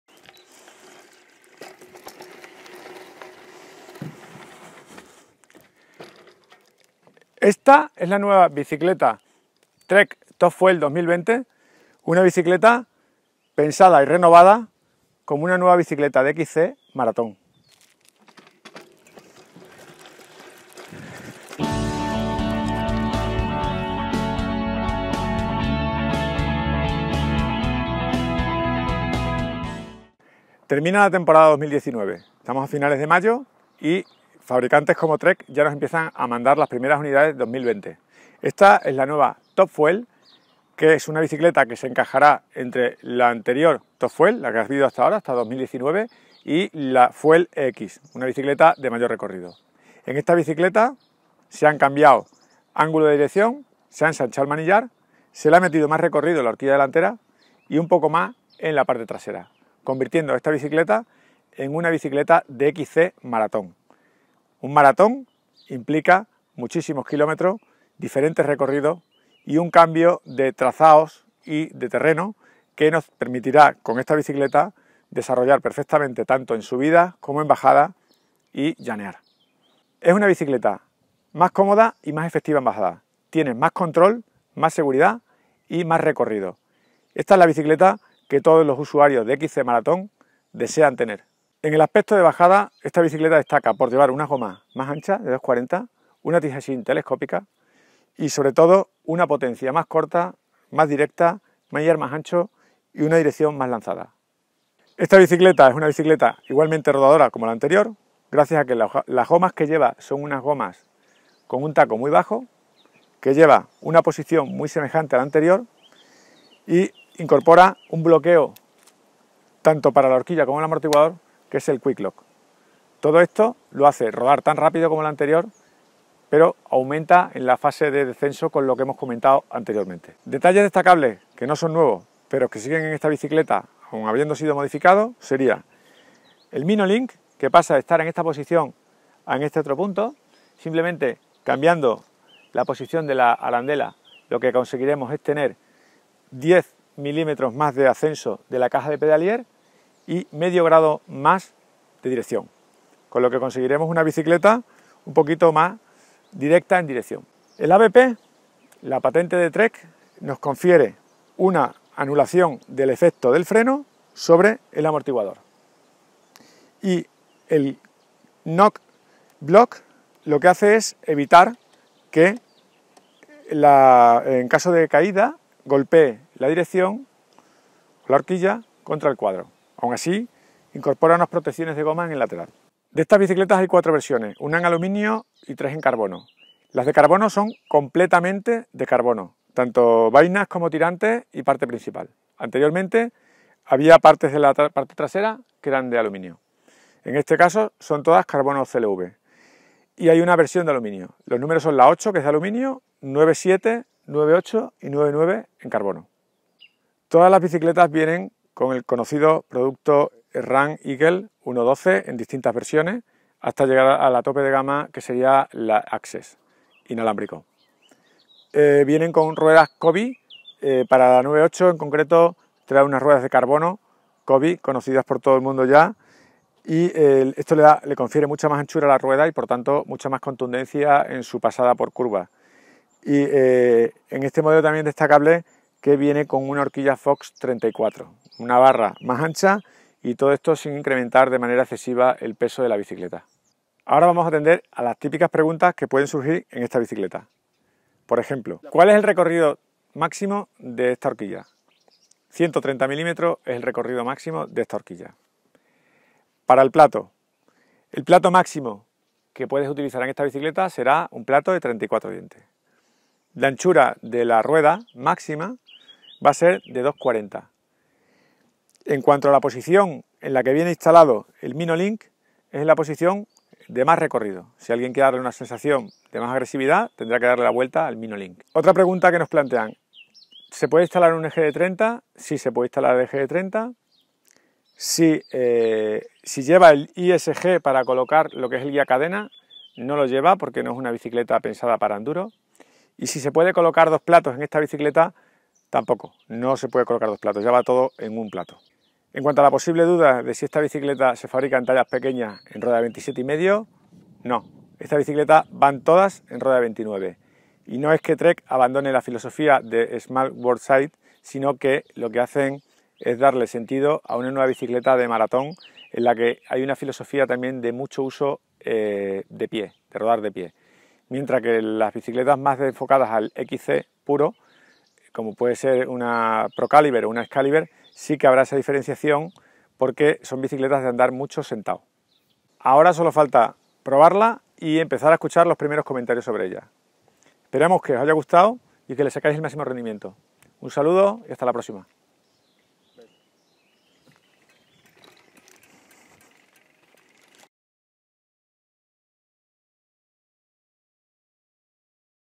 TREK TOP FUEL 2020: la nueva doble suspensión de Cross Country / XC Marathon | ANÁLISIS
El fabricante de Wisconsin  🇺🇸 presenta un modelo mid season de la TOP FUEL.
La versión 2020 sufre importantes modificaciones :
▶ Geometría totalmente renovada,
▶ Posición de conducción  tipo "Forward"
▶ Nuevo Mino-Link,
▶ Accesorios para el "MTB actual"...

Una prueba más de que la evolución del mountain bike hacia un estilo de montar cada vez más técnico y de menos kilómetros, es imparable.
Un detallado análisis en el vídeo.

¿CUÁNTO CUESTA LA TOP FUEL 2020?

¿Quieres que hablemos de algún tema o producto?
Deja tu comentario, y te contestamos.